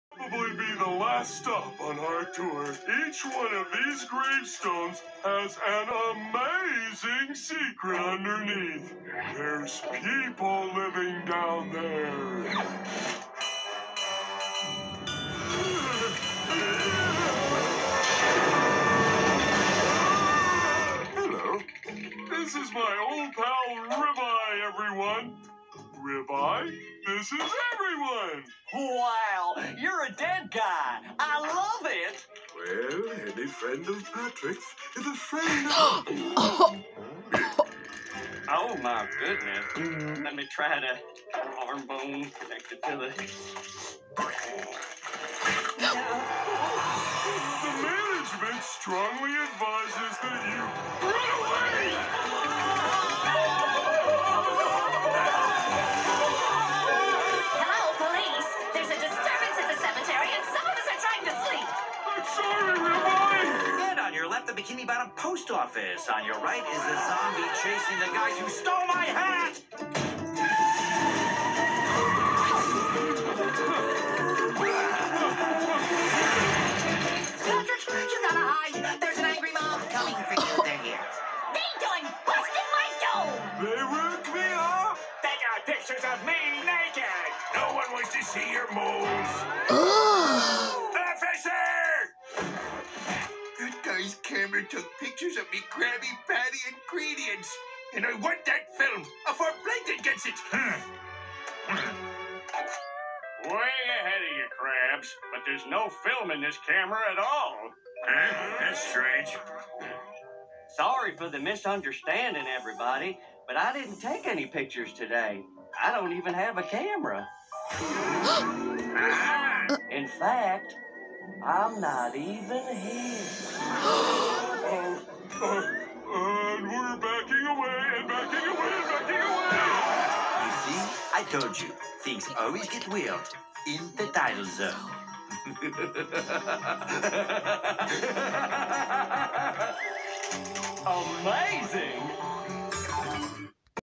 Everyone Angrily Chases After Them and He Disappears At The End 😮
Patrick and the photographer are disturbing the cemetery then they are being chased by everyone and the photographer suddenly disappears at the end